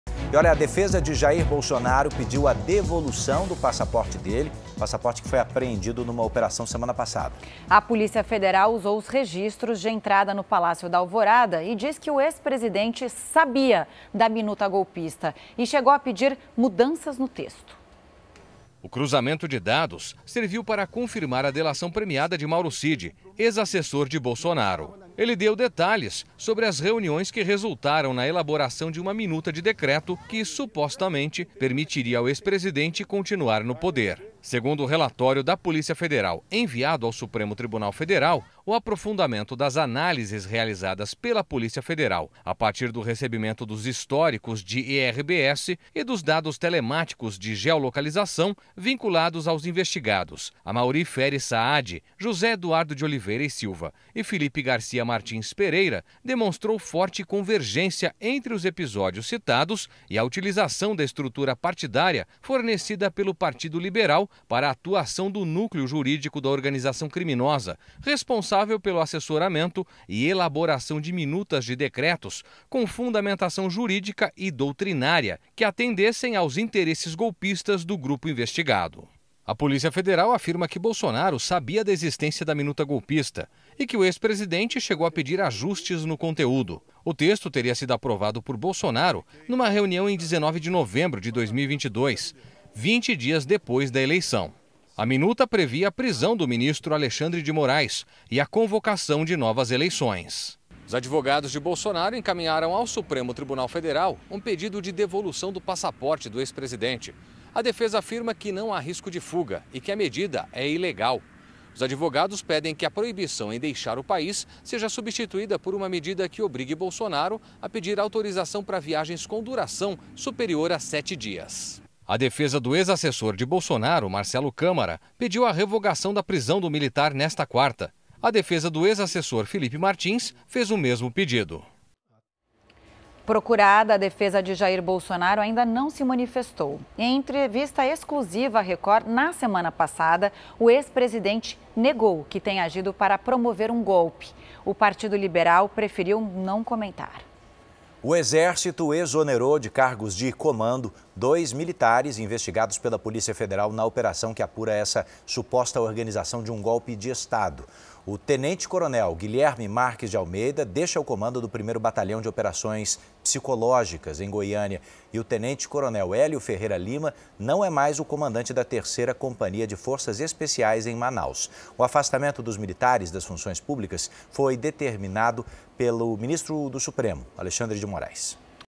Defesa de Bolsonaro pede devolução de passaporte apreendido após operação
A defesa de Jair Bolsonaro pediu a devolução do passaporte do ex-presidente, apreendido numa operação na semana passada. A Polícia Federal usou os registros de entrada no Palácio da Alvorada e diz que o ex-presidente sabia da minuta golpista e chegou a pedir mudanças no texto. Exército exonera de cargos de comando militares suspeitos de participação em trama golpista.